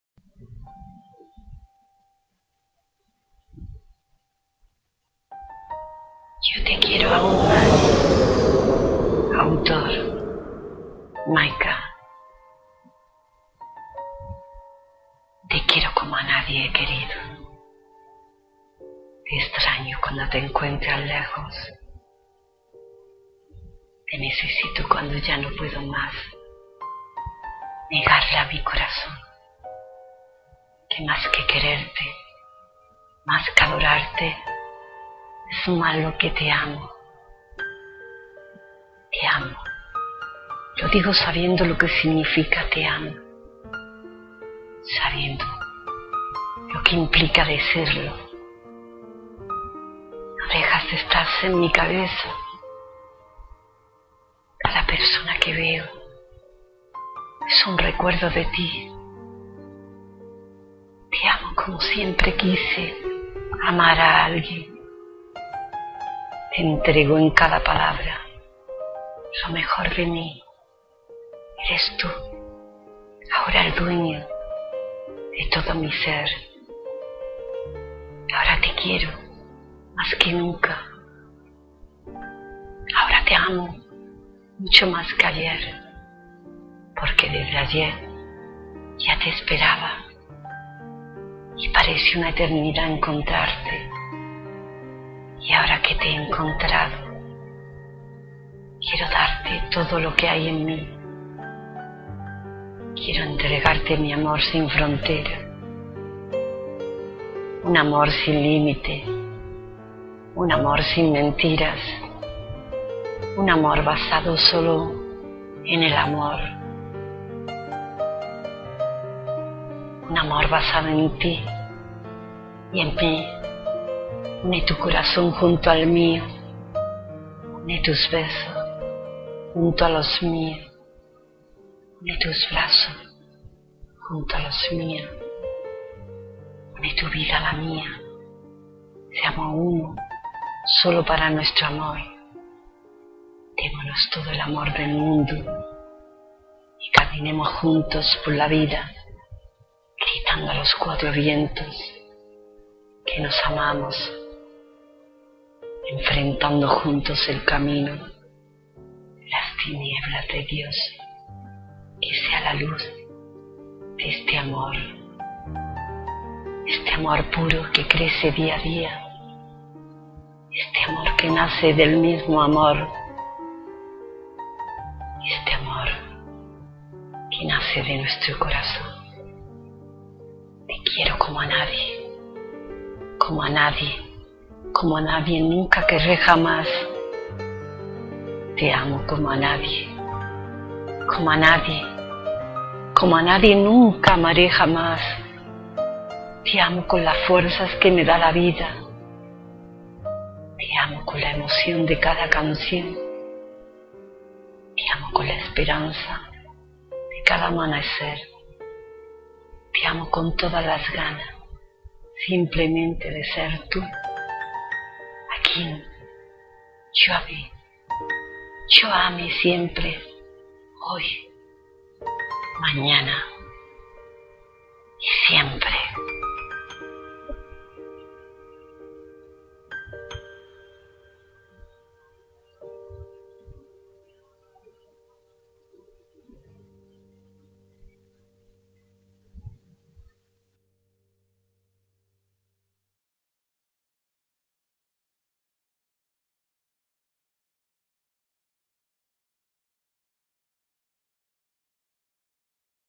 Yo te quiero mas aun cada dia - Mayka - && Declamado Mayka el sentir del Poeta &&
Aqui os dejo este bello poema mio que os dedico ademas de mi agradecimiento siempre un abrazo muchas gracias.